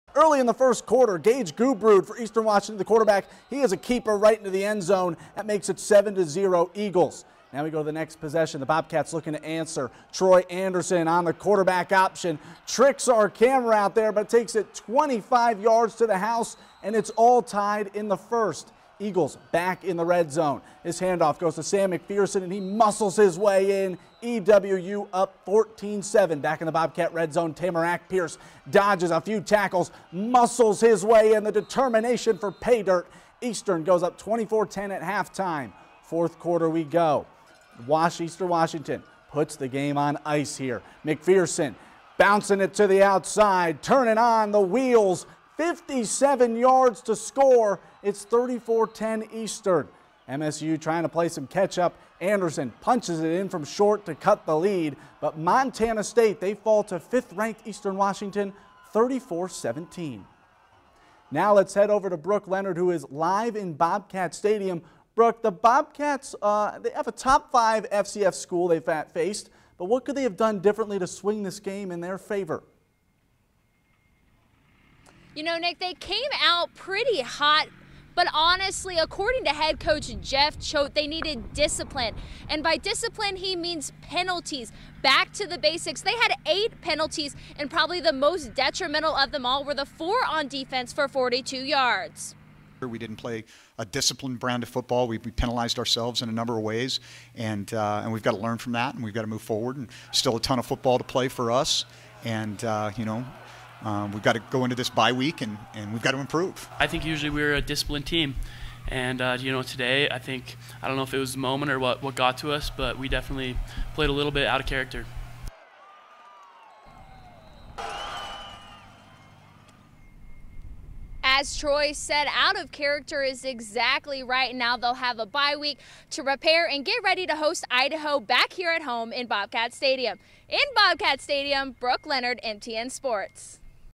MSU falls to #5 Eastern Washington
The Montana State Bobcats move to 1-1 in Big Sky play after their lose to Eastern Washington.